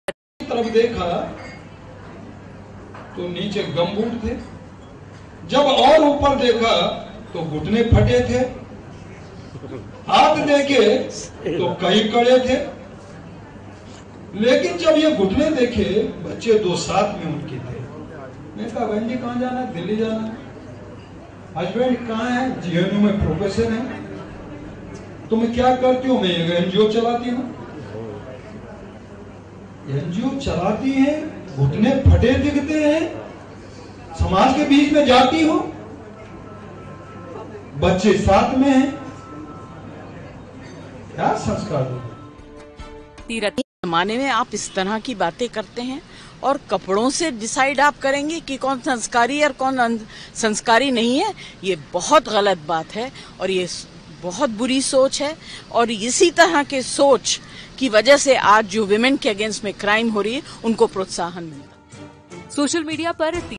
'फटी जींस' पर घिरे तीरथ रावत, अमिताभ बच्चन की पत्नी ने कहा CMसाहब सोच बदलो, तभी देश बदलेगा
उत्तराखंड के नए मुख्यमंत्री तीरथ सिंह रावत द्वारा महिलाओं के पहनावे को लेकर जो बयान दिया गया है, उसपर विवाद बढ़ गया है. सोशल मीडिया पर इस बयान पर बहस छिड़ गई है, तो वहीं राजनीतिक प्रतिक्रियाओं का भी अंबार लग गया है. तृणमूल कांग्रेस की सांसद महुआ मोइत्रा ने तीरथ सिंह रावत के बयान पर तीखा हमला बोला है. महुआ मोइत्रा के अलावा प्रियंका चतुर्वेदी ने भी तीरथ सिंह रावत पर निशाना साधा है. टीएमसी सांसद महुआ मोइत्रा ने ट्विटर पर लिखा कि उत्तराखंड CM कहते हैं, ‘जब नीचे देखा तो गम बूट थे और ऊपर देखा तो... एनजीओ चलाती हो और घुटने फटे दिखते हैं?’ CM साहब, जब आपको देखा तो ऊपर-नीचे-आगे-पीछे हमें सिर्फ बेशर्म-बेहूदा आदमी दिखता है.
Uttarakhand CM Tirath Singh Rawat jeans Speech.
CM Tirath Singh Rawat News latest